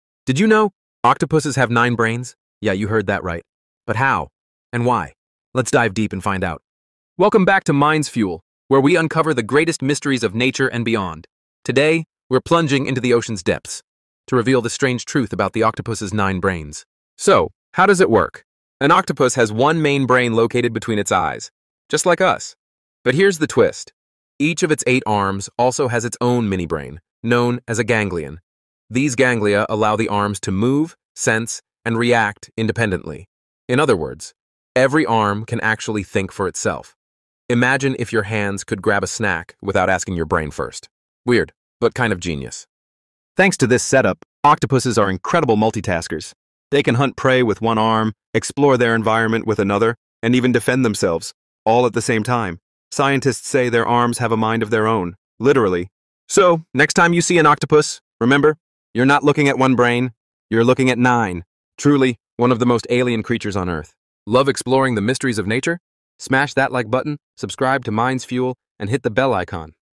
Do You Know an Octopus Has Nine Brains? 🧠🐙 | Mind-Blowing Fact
Did you know that an octopus has not one, not two, but NINE BRAINS? 🤯 Each arm has a mini-brain that controls movement independently!